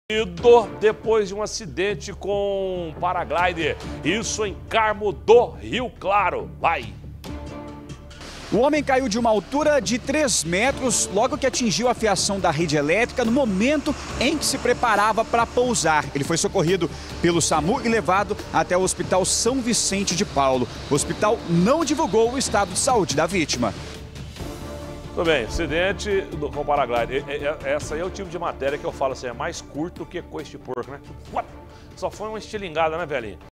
Idoso ferido após acidente com paraglider em Carmo do Rio Claro
ACIDENTE COM PARAGLIEDER: IDOSO FERIDO | Um idoso ficou ferido após um acidente com paraglider em Carmo do Rio Claro.